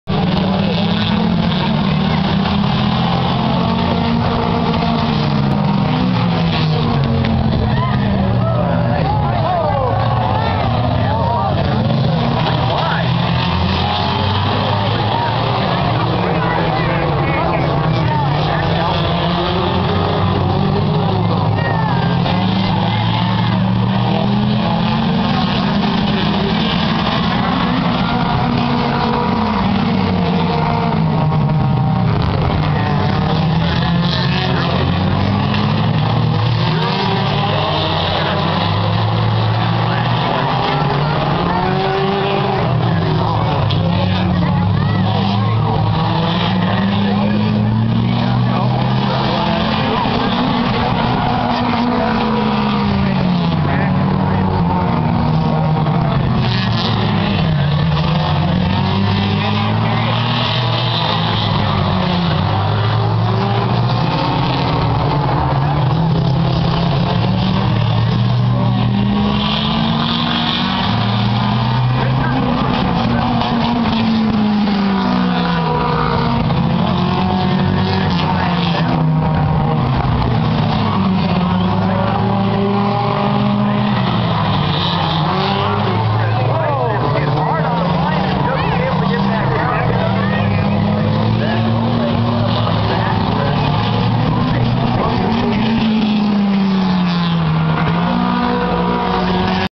South Sound Speedway Bumblebees
Just some footage from the local speedway taken yesterday, this is footage of the Bumblebees.